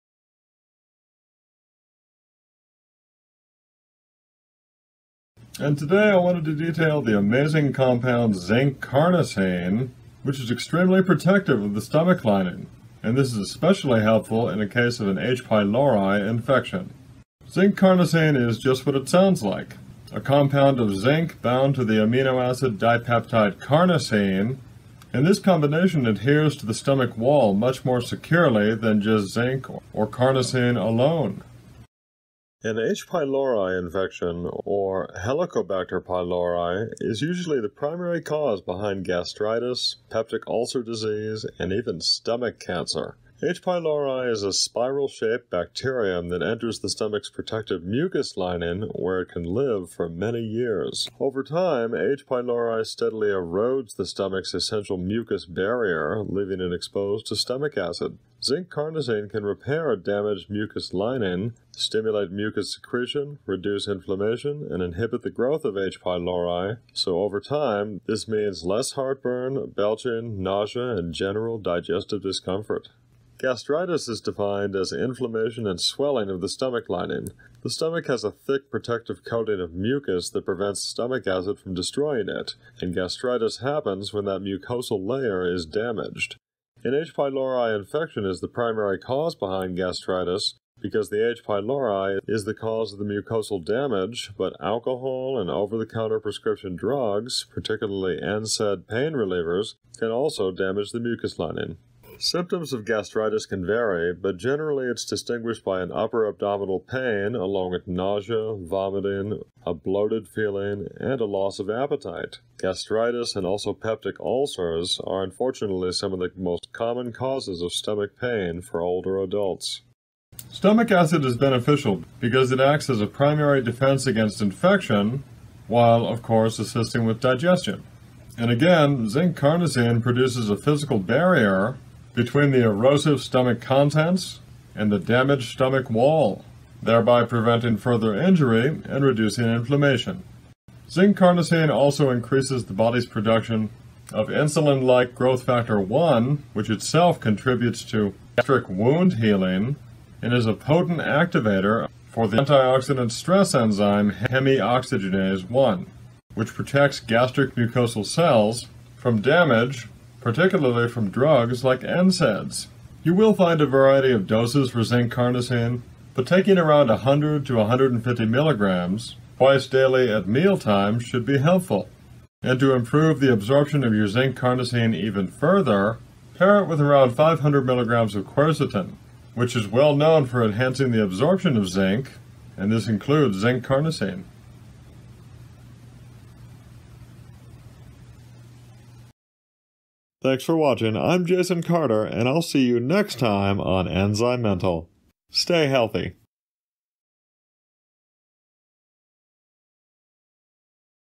What is Zinc Carnosine?
Today I detail Zinc Carnosine, which is extremely protective of the stomach lining, and can adhere to the stomach wall much more securely than zinc or carnosine alone.  I also tell you an easy way to improve the absorption of your zinc carnosine.